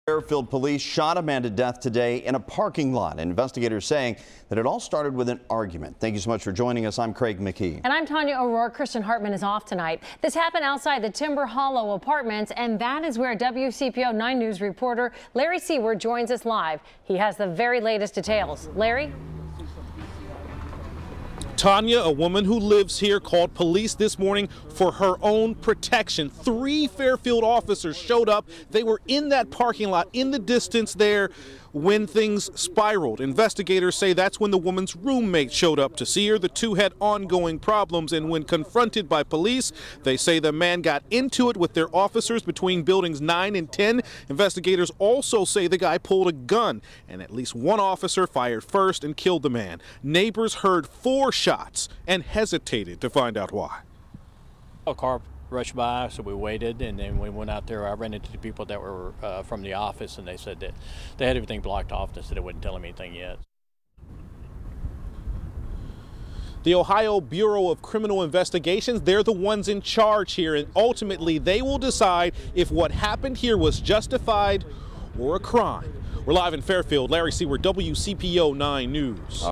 Suspect dead in Fairfield officer-involved shooting, officers on leave
Police shot a man to death Friday afternoon while responding to a call for protection at the Timber Hollow Apartments near Dixie Highway.